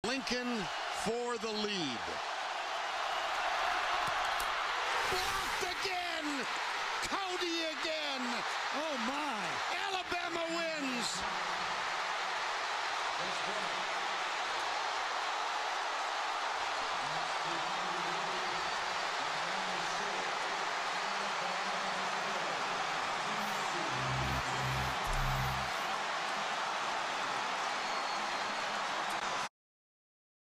62 Days till Alabama Football Terrence Cody made the play of the year in 2009 vs Tennessee
62 Days till Alabama Football 🐘🔥

Never forget when Terrence Cody absolutely made the play of the year in 2009 vs Tennessee and Lane Kiffin 🙂🔥